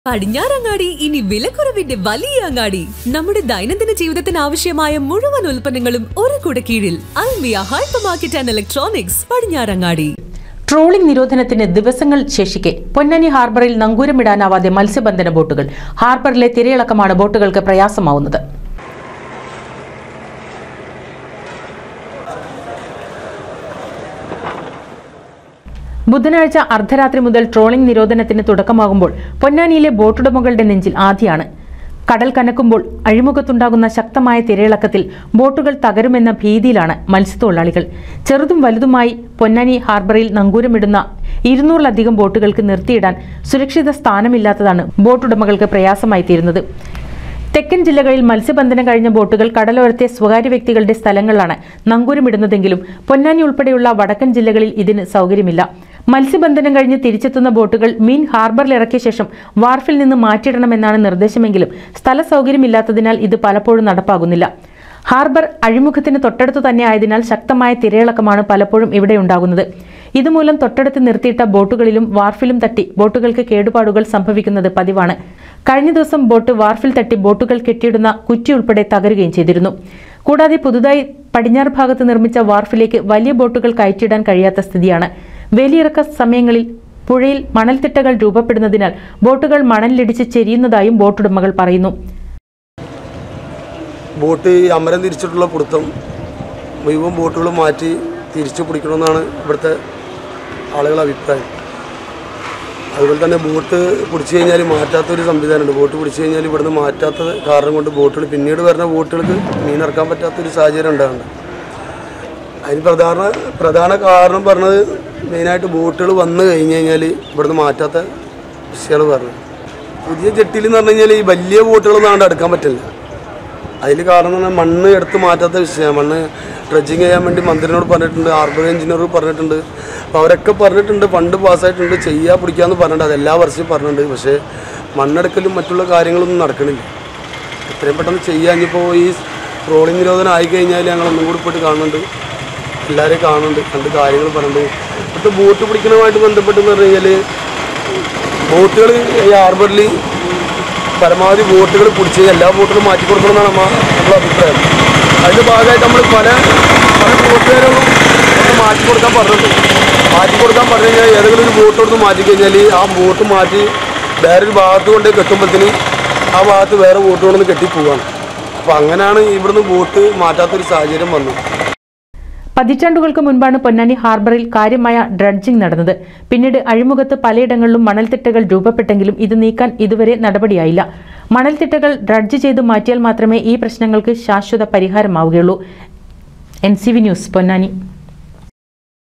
ട്രോളിങ് നിരോധനത്തിന് ദിവസങ്ങള്‍ ശേഷിക്കെ ഹാര്‍ബറില്‍ നങ്കൂരമിടാനാവാതെ മത്സ്യബന്ധന ബോട്ടുകള്‍
ട്രോളിങ് നിരോധനത്തിന് ദിവസങ്ങള്‍ ശേഷിക്കെ പൊന്നാനി ഹാര്‍ബറില്‍ നങ്കൂരമിടാനാവാതെ മത്സ്യബന്ധന ബോട്ടുകള്‍. ഹാര്‍ബറിലെ തിരയിളക്കമാണ്  ബോട്ടുകള്‍ക്ക്  പ്രയാസമാവുന്നത്